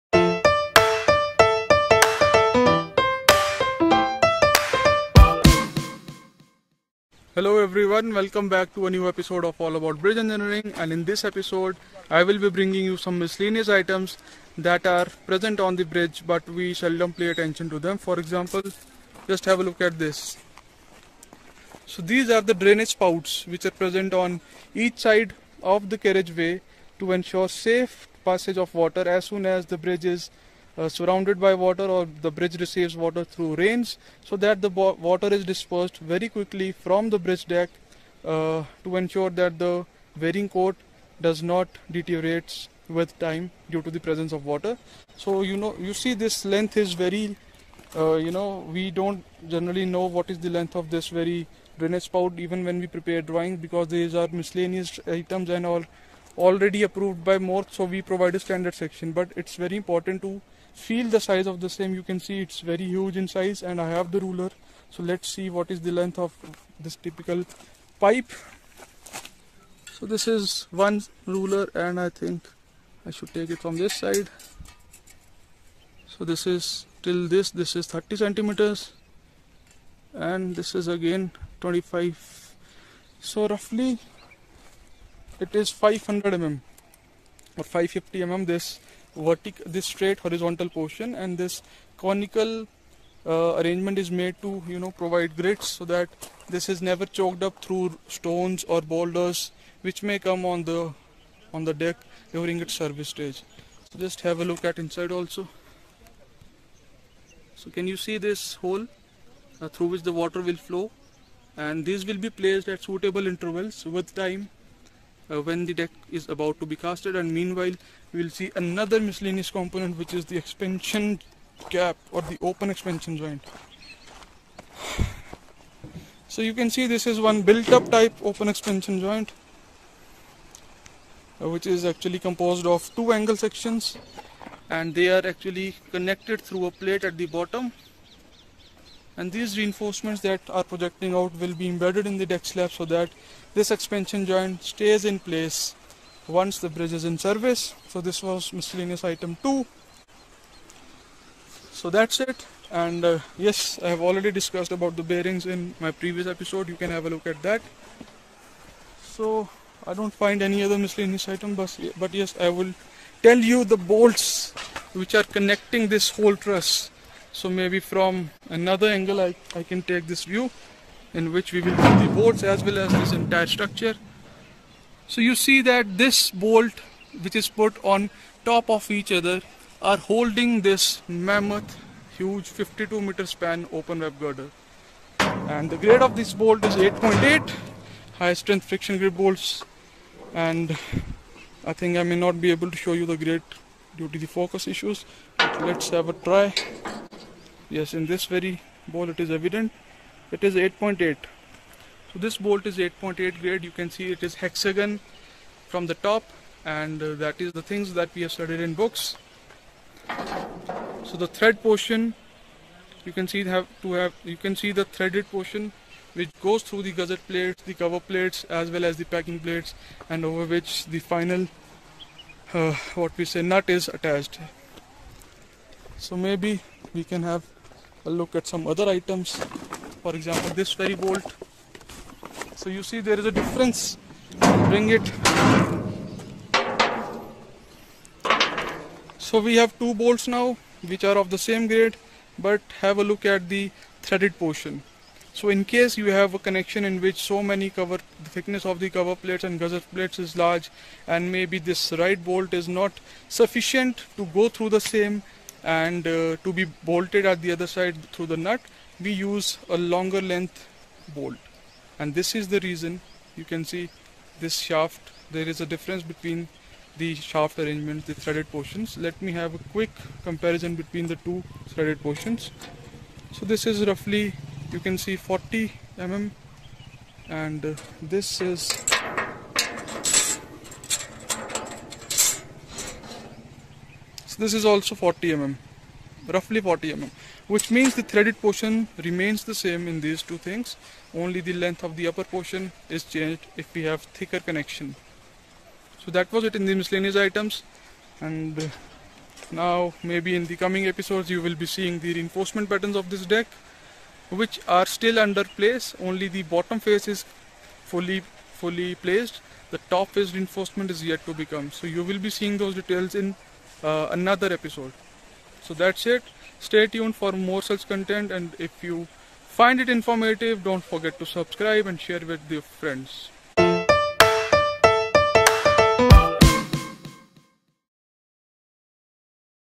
EPISODE 7 | MISCELLANEOUS ITEM-DRAINAGE SPOUT, BOLTS, EXPANSION JOINT | JOURNEY OF A TRUSS BRIDGE
In this episode, miscellaneous items present on the bridge like the drainage spout, expansion joints, bolts etc., are discussed which also play an important role in ensuring proper serviceability of the bridge.

Drainage Spout : These are pipes installed on the edge of carriageway as per the cross slope direction to drain off water accumulated on the bridge deck due to rainfall. A practical onsite drainage spout is described in the episode and it is noted that the straight pipe portion has approximate length of 500mm. Sufficient screen plates are also provided on the inlet side to stop vehicle tires and boulders from damaging or choking the spout.

Expansion Joint : In the structure under consideration, a built up open joint composed of two angle sections with dowel reinforcement are present and same have been very briefly discussed.

HSFG Bolts: The heavy bolts connecting the entire 52m span and holding individual members as a single unit are discussed in detail along with the comparison between long and small bolts. It is observed that the shank length varies for bolts having different total length. However, the length of the threaded portion remains same.

More details of the same will be shared as the structure proceeds towards completion.